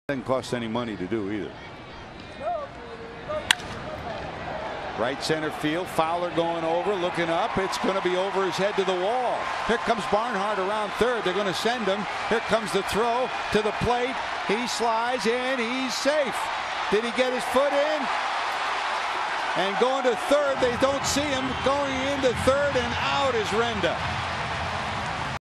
CHC@CIN: Renda plates Barnhart with a double in 4th
Tony Renda drills a double to right-center field, scoring Tucker Barnhart to extend the Reds' lead to 4-2 before being cut down at third